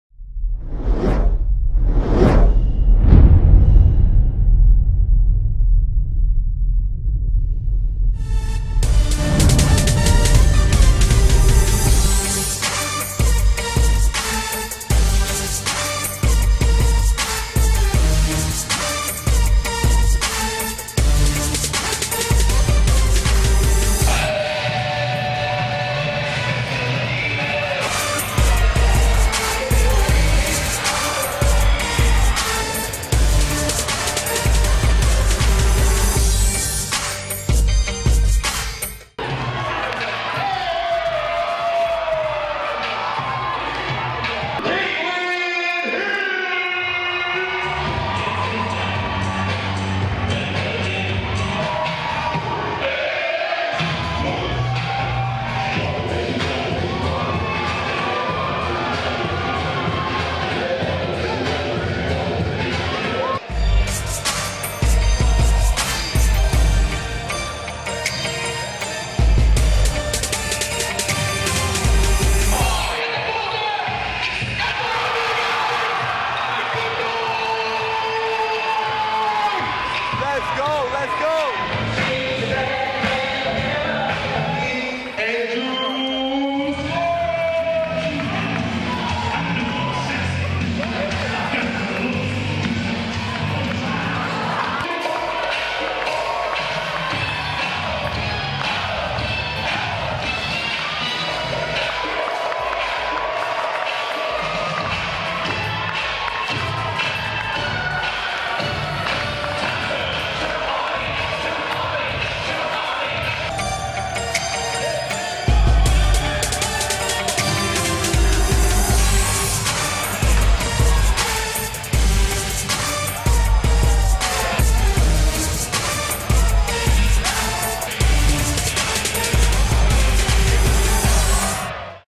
Midnight Madness 2012
Sights and Sounds from the first ever Division I Midnight Madness at Bryant University.